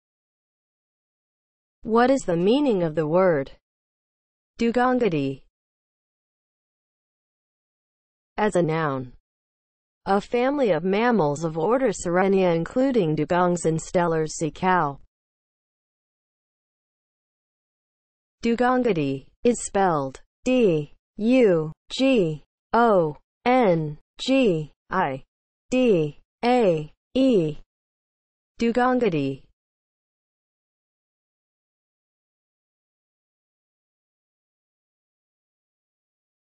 What is the meaning of the word DUGONGIDAE?
Definition and spelling of the word DUGONGIDAE.

(00:00) Intro
(00:07) As a noun
(00:17) Spelling

As a noun (00:07):
• a family of mammals of order Sirenia including dugongs and Steller's sea cow